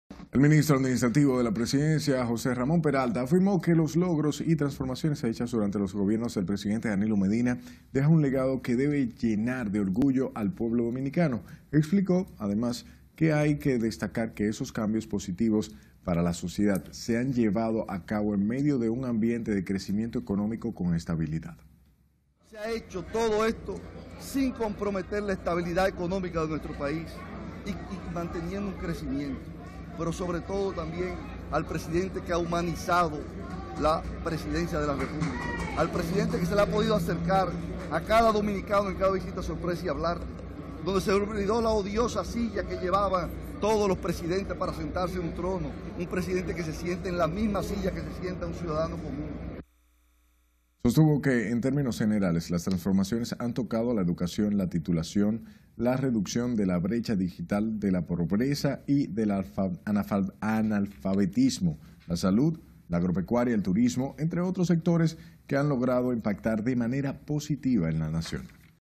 Ministro Administrativo: Gobierno deja legado y orgullo al país
El Ministro Administrativo de la Presidencia, José Ramón Peralta, afirmó que los logros y transformaciones hechas durante los gobiernos del presidente Danilo Medina deja un legado que debe llenar de orgullo al pueblo dominicano.